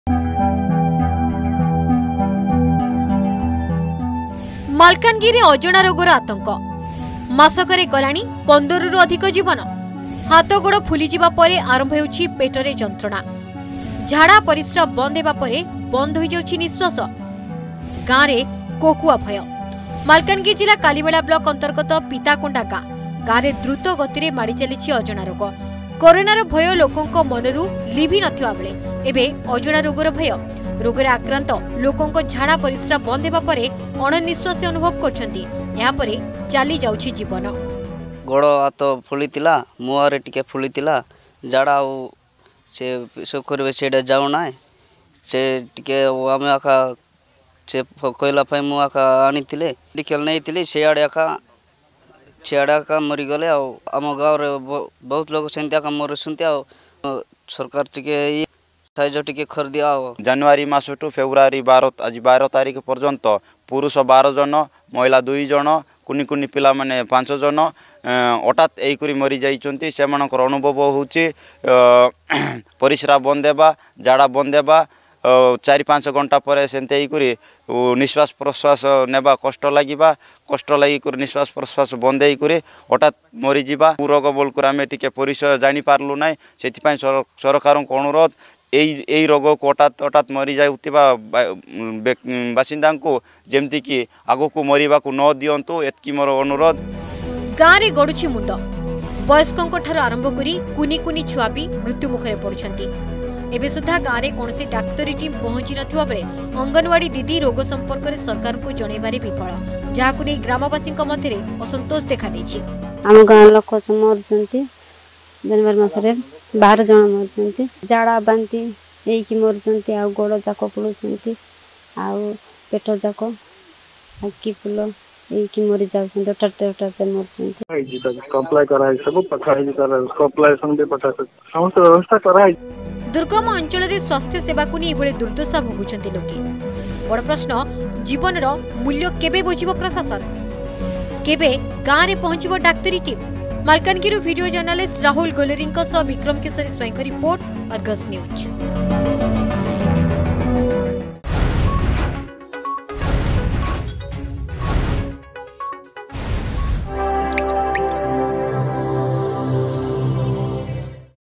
Unknown Life-Threatening Disease Terrorizes Villagers, 15 People have died in a month in Malkangiri
Unknown Life-Threatening Disease Terrorizes Villagers, More than 15 lives are lost in a month, Symptoms of the Unknown disease include Swelling of the hands and feet with abdominal pain. Pitakonda villagers under Kalimela block of Malkangiri district have fallen prey to unknown diseases. While the fear of corona has not gone yet from the minds of the people, now the fear of unknown diseases.